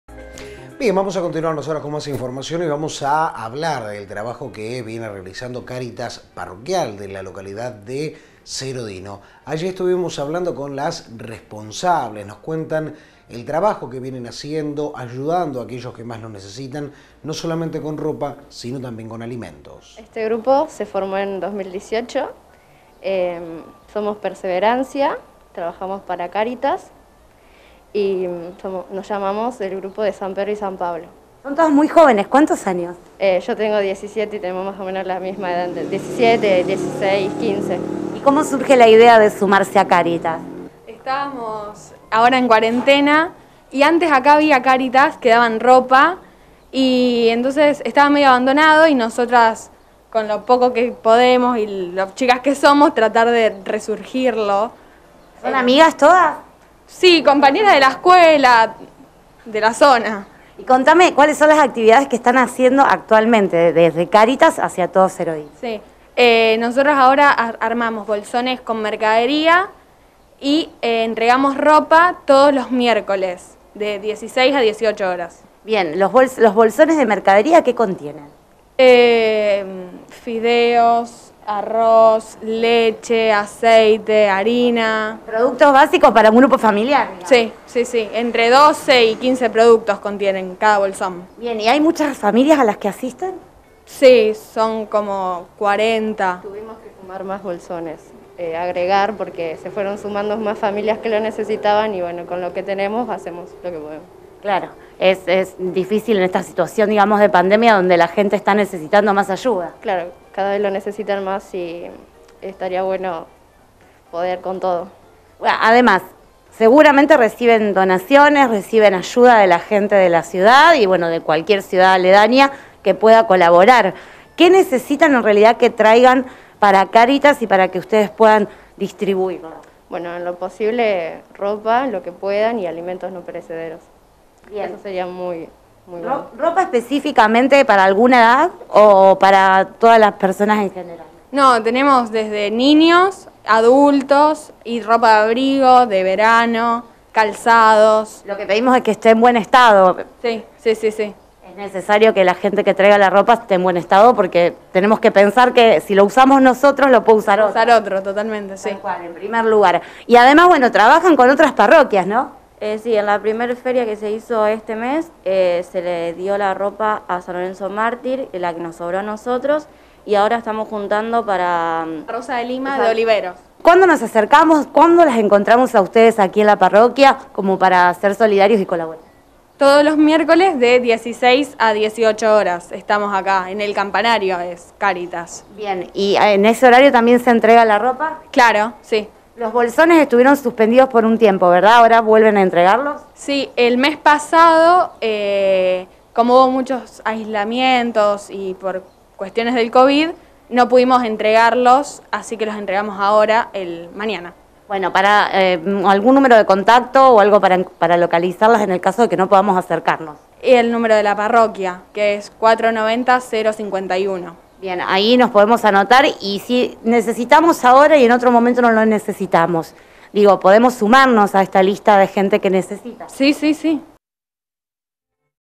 Actividades de Cáritas Parroquial Serodino
Unas cuarenta familias son asistidas periódicamente por las jóvenes integrantes de la agrupación Cáritas Parroquial Serodino.